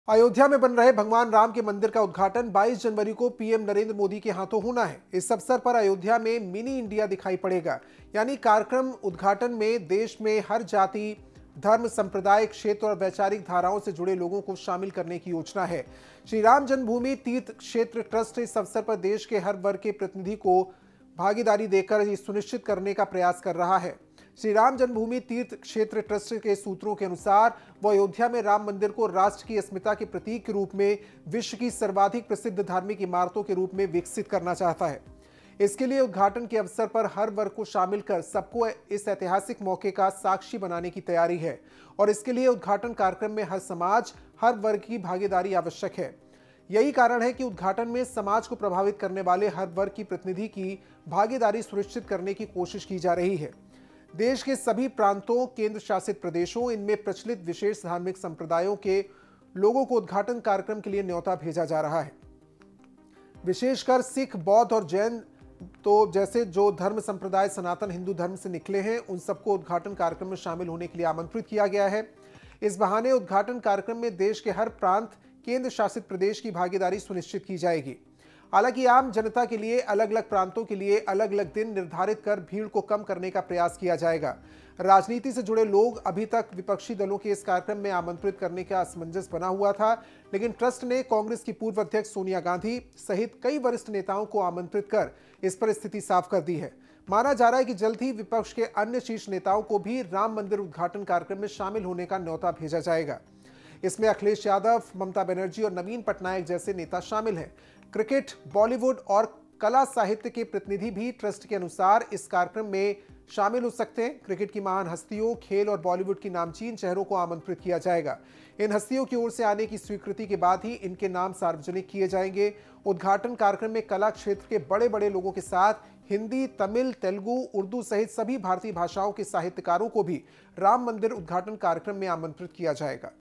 Ayodhya Ram Mandir में दिखेगा 'Mini India', उद्घाटन में हर वर्ग के लोगों को शामिल करने की कोशिश
श्री राम जन्मभूमि तीर्थ क्षेत्र ट्रस्ट सूत्रों के अनुसार, वह अयोध्या के राम मंदिर को राष्ट्र की अस्मिता के प्रतीक के रूप में विश्व की सर्वाधिक प्रसिद्ध धार्मिक इमारतों के रूप में विकसित करना चाहता है। इसके लिए उद्घाटन के अवसर पर हर वर्ग को शामिल कर सबको इस ऐतिहासिक मौके का साक्षी बनाने की तैयारी है।